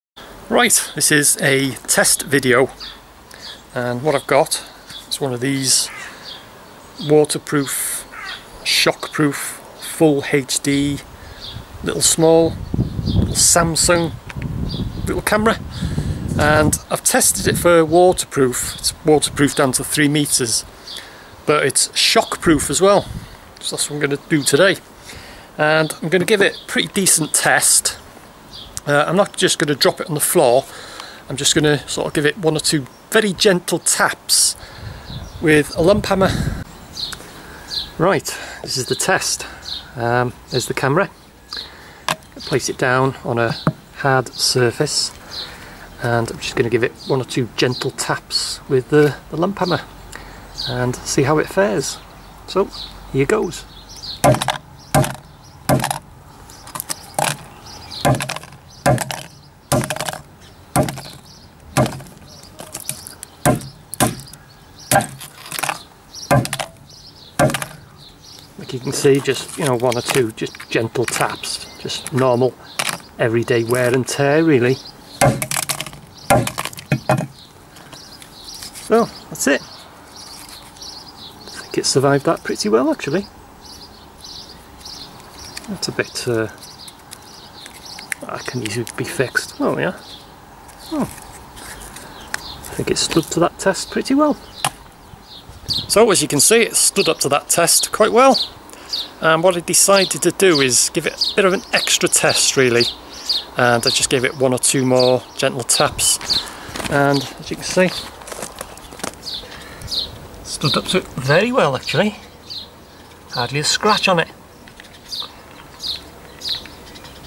HD Camera ... shock test
Subjecting my Samsung HD Camera to some everyday wear and tear